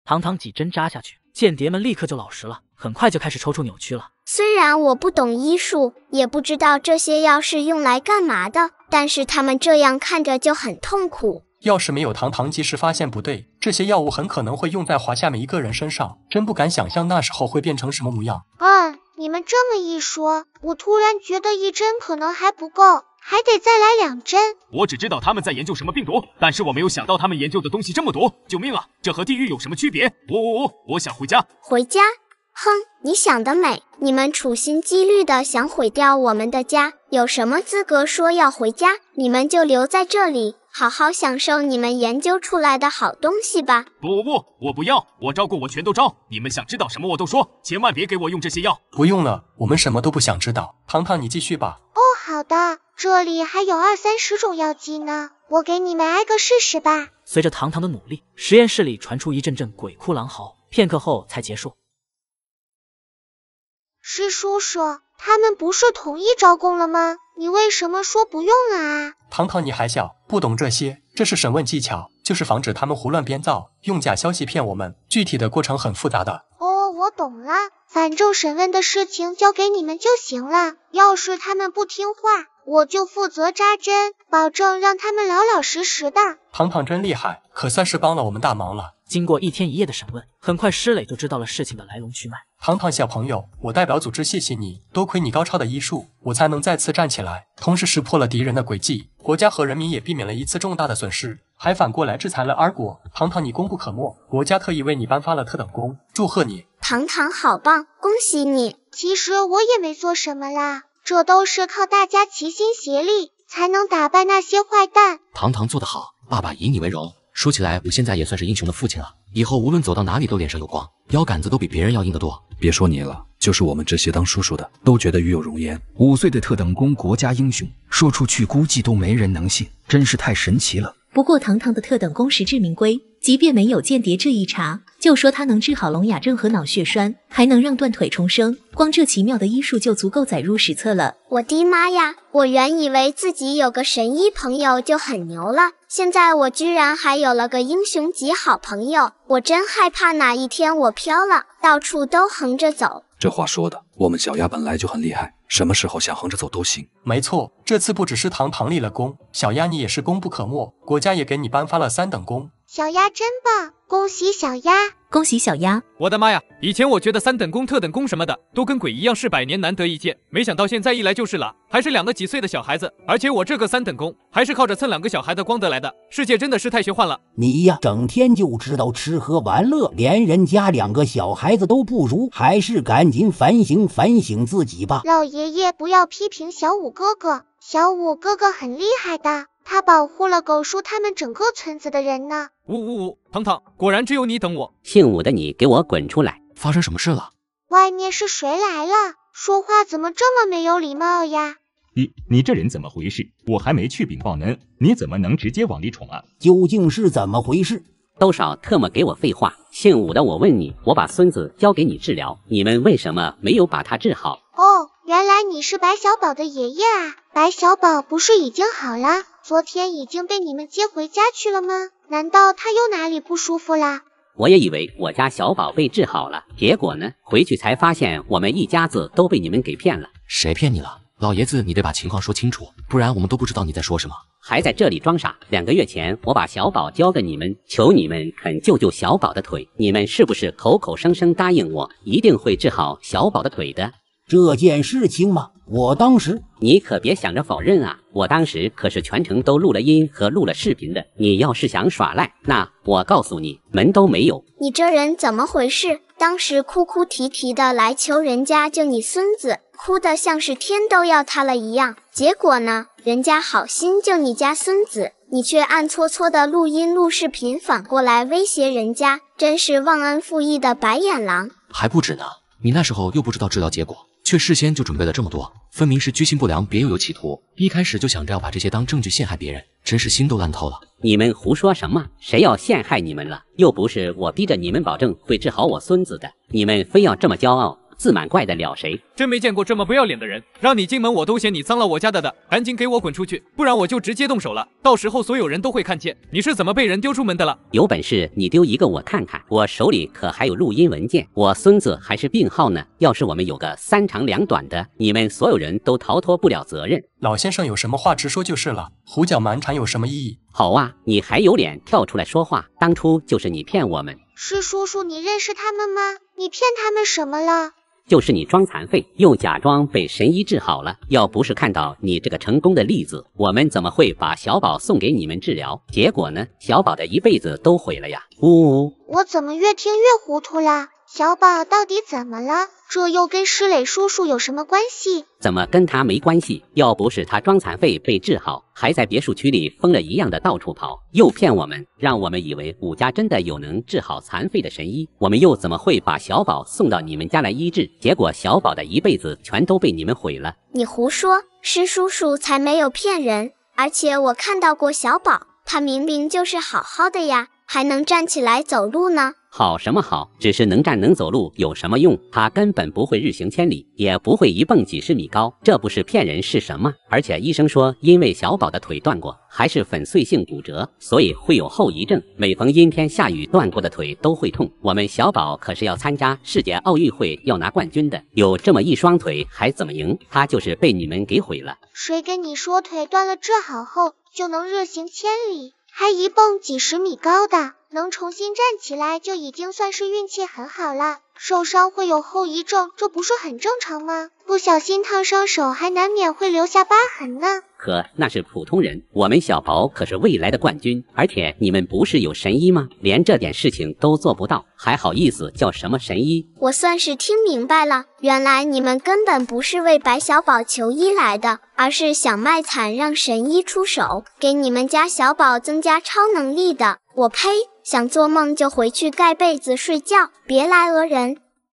【萌娃糖糖】EP 270丨糖糖對間諜們用酷刑，僅僅紮了一針，對方竟全招了！ 【阿飛】
糖糖對間諜們用酷刑，僅僅紮了一針，對方竟全招了！

大家好我是阿飛
歡迎大家來看我的沙雕動畫
也很高興能給大家帶來樂趣